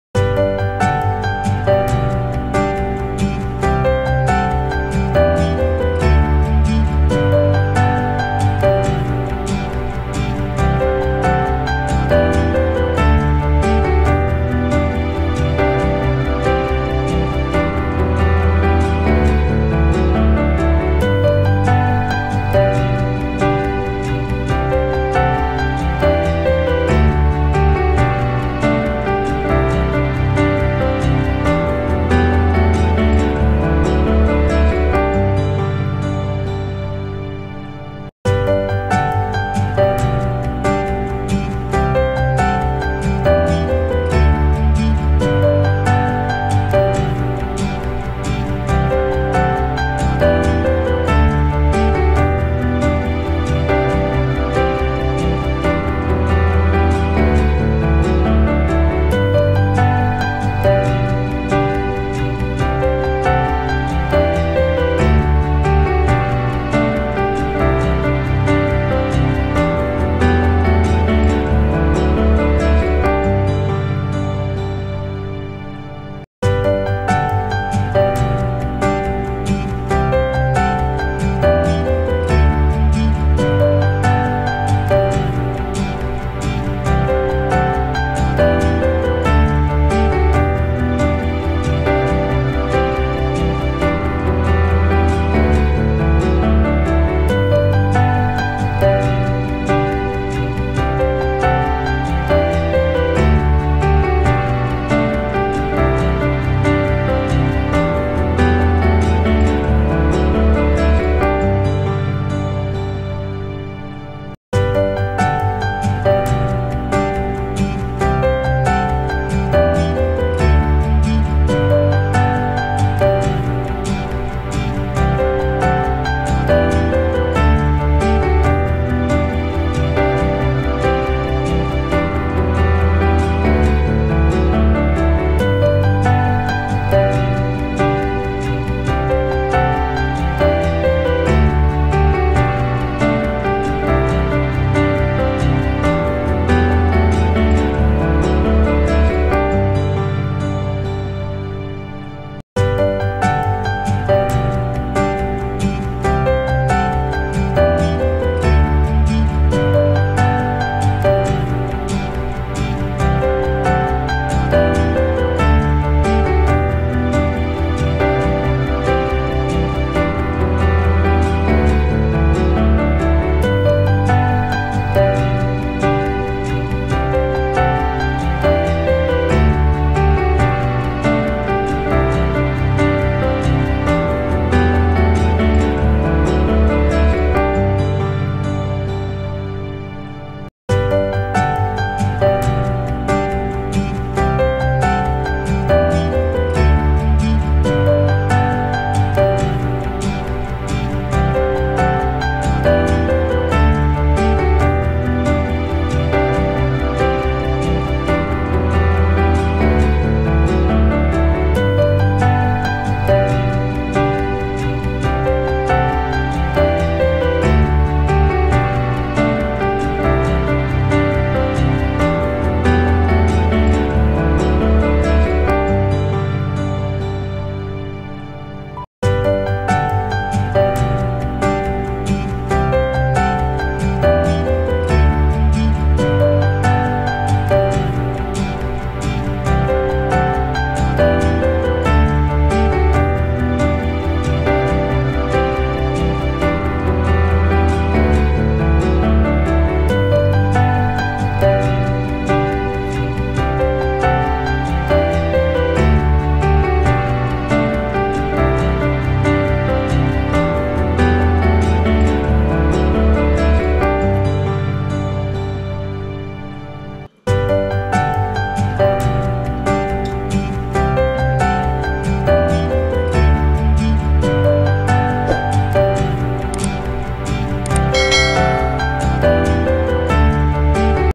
Cassava Cake | British-Pinay Kitchen
Ingredients:

1/2 kg grated cassava
1 cup sugar
1/4 tsp salt
1 large egg
1 cup macapuno strips
Butter/oil to grease the pan

Toppings:
1can coconut milk
1 1/2 Tbsp all purpose flour
1cup condensed milk